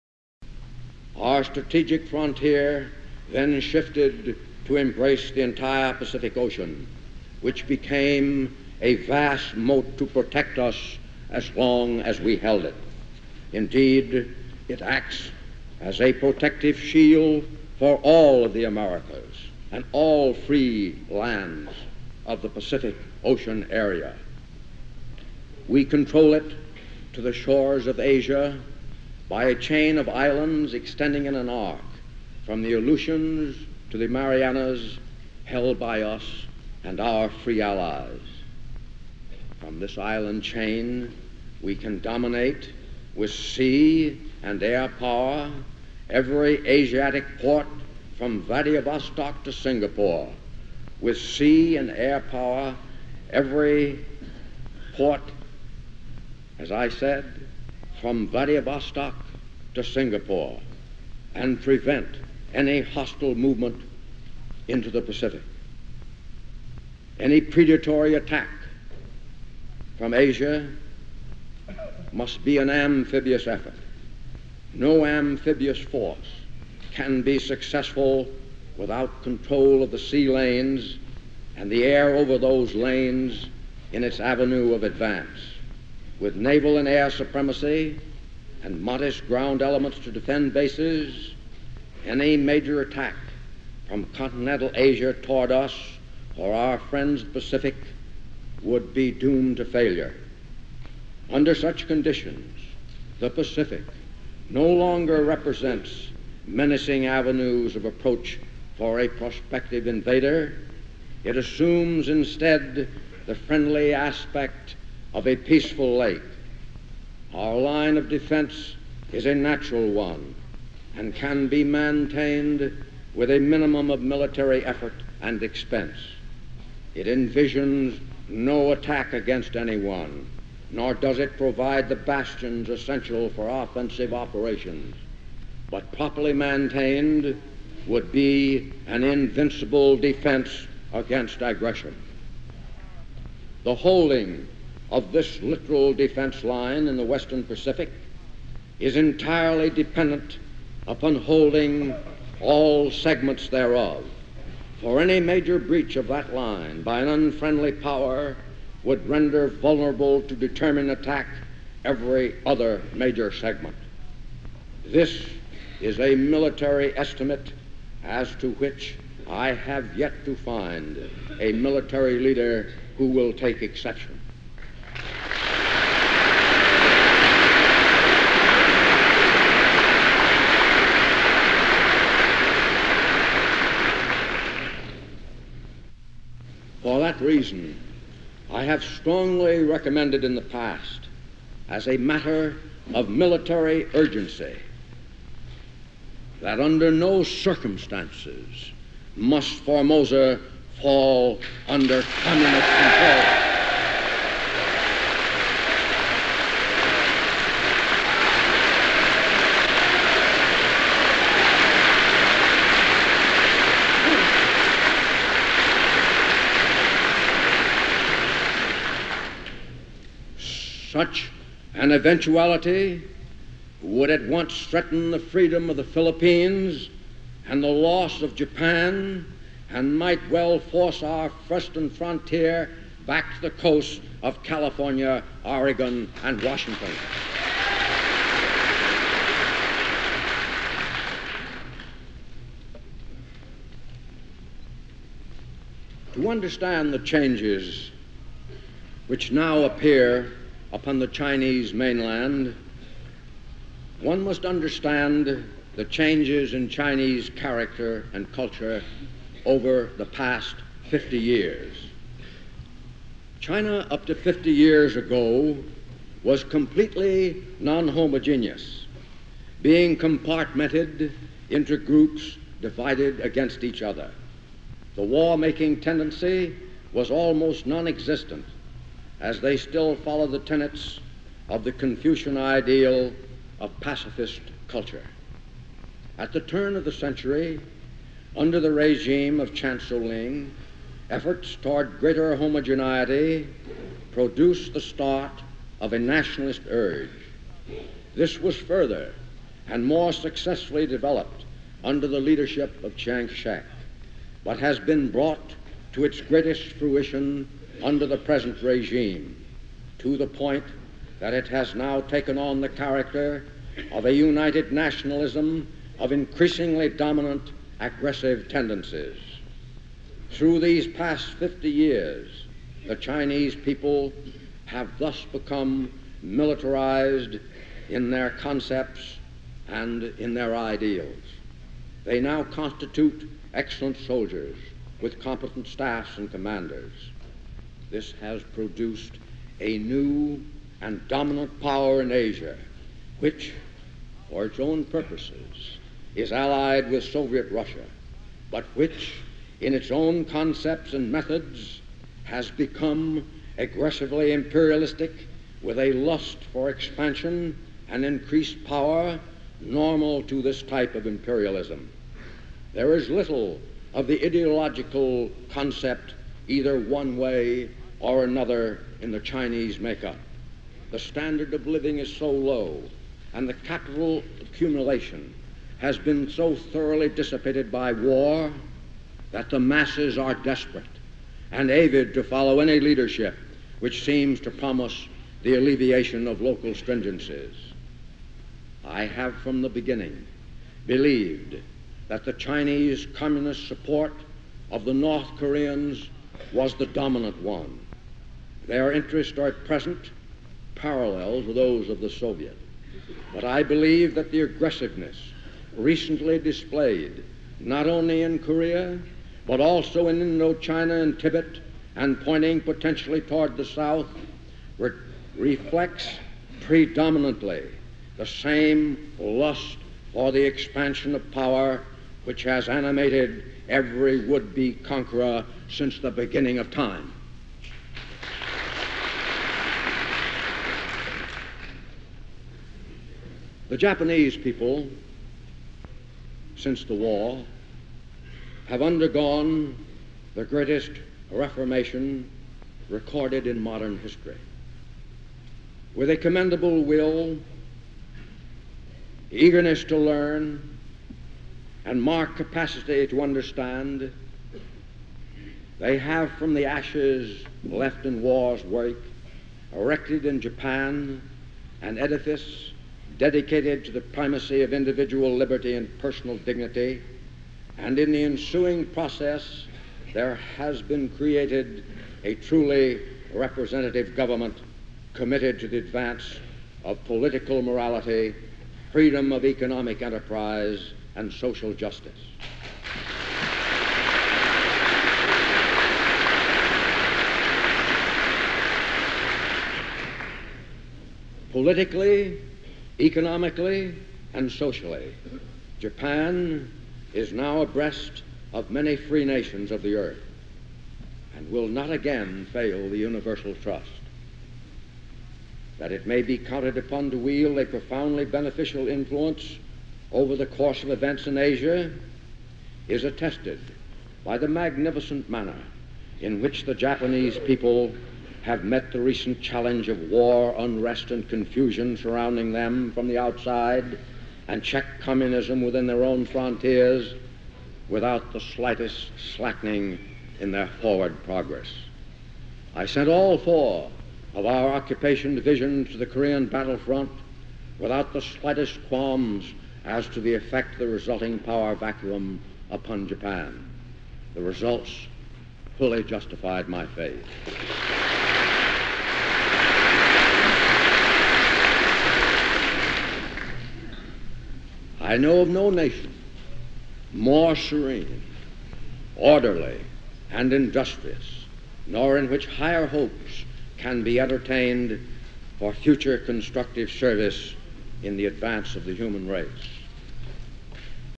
Douglas MacArthur - Address to Congress (1951) Part 2 of 4
Douglas General MacArthur
Address to the Joint Meeting of the U. S. Congress

Total Length approximately 36 minutes.  Divided into four parts, each about 9 minutes.

General Douglas MacArthur, former commander of Allied forces, first in World War II and then in Korea, gives his farewell address before the joint meeting of Congress on April 19, 1951. President Truman relieved MacArthur of his command over differences in opinion on strategy in Korea. In a final Address to Congress, MacArthur defends his conduct of the Korean War. "Old Soldiers never die," he famously says, "they just fade away."